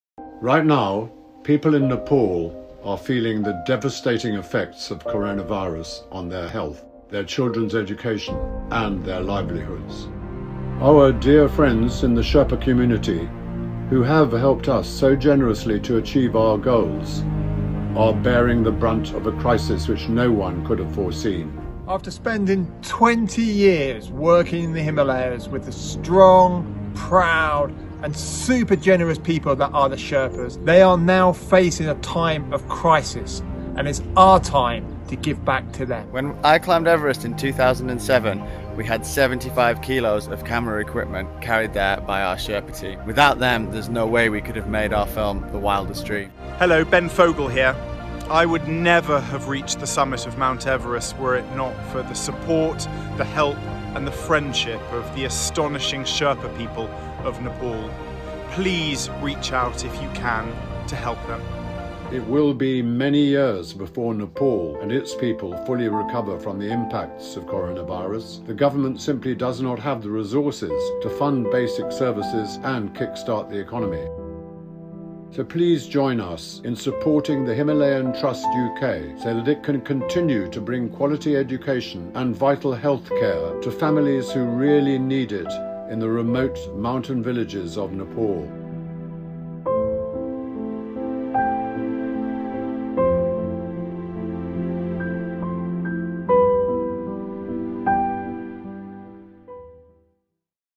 Himalayan Trust UK - Helping Nepal short clip
Reasons why it is time to give back to the people in the remote mountain communities of Nepal in these times of COVID-19. and sign up to our mailing list

Right now, people in Nepal are feeling the devastating effects of coronavirus on their health, their children’s education and their livelihoods. As is so often the case, the people hit hardest are those who have the least. Our dear friends in the remote mountain communities, who have helped many so generously to achieve their goals, are bearing the brunt of a crisis which no one could have foreseen.

Hear from Kenton Cool, Leo Houlding, Sir Ranulph Fiennes and Ben Fogle why it is time to give back to the people of Nepal.

It will be many years before Nepal will fully recover from the impacts of coronavirus – the government simply does not have the resources to fund basic services and kick-start the economy. Please help us to continue to bring quality education and improved health services to families who really need it in the remote mountain villages of Nepal.

Himalayan Trust’s founder Sir Edmund Hillary said that his greatest achievement was not climbing Everest, but helping the people of the Himalayas.

They need our help now more than ever.